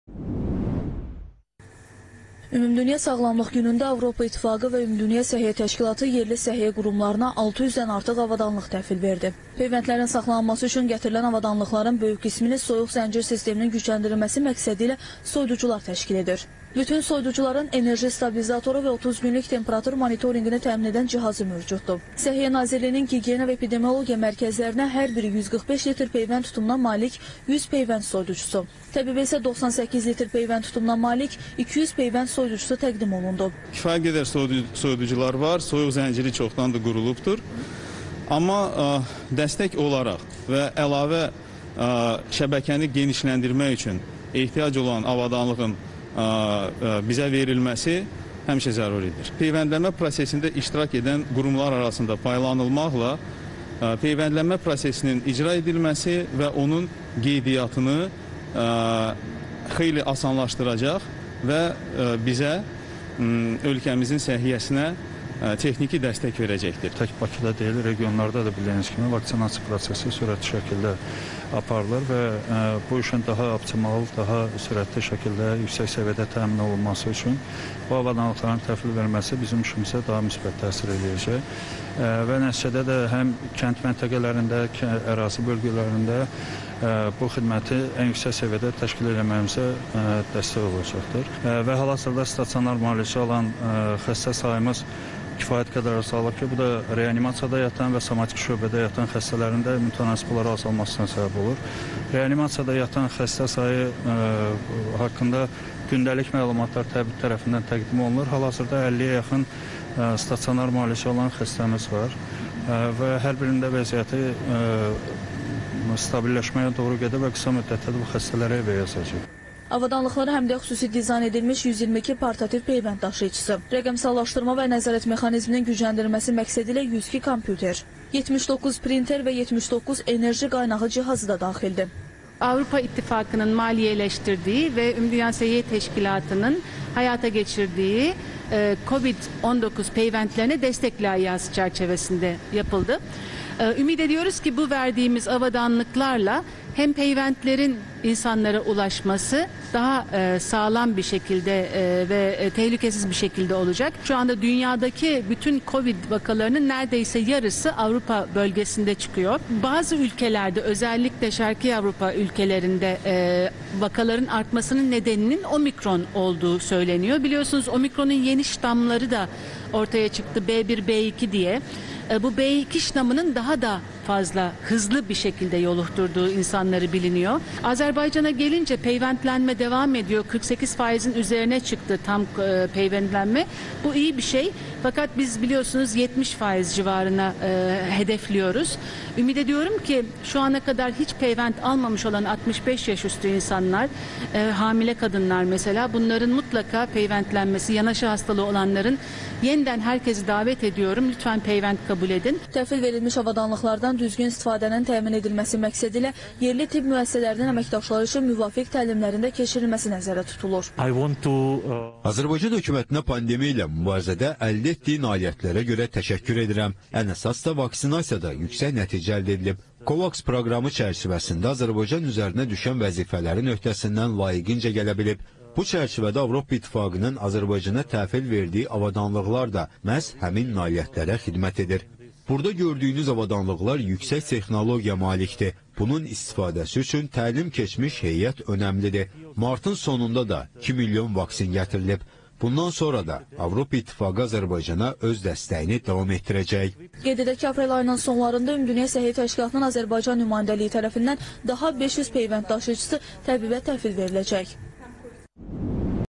Ölkəyə 600-dən çox tibbi avadanlıq gətirilib
Tədbir çərçivəsində Səhiyyə Nazirliyinin Gigiyena və Epidemiologiya Mərkəzləri hər biri 145 litr peyvənd tutumuna malik 100 peyvənd soyuducusu ilə təmin edilib.

Əlaqə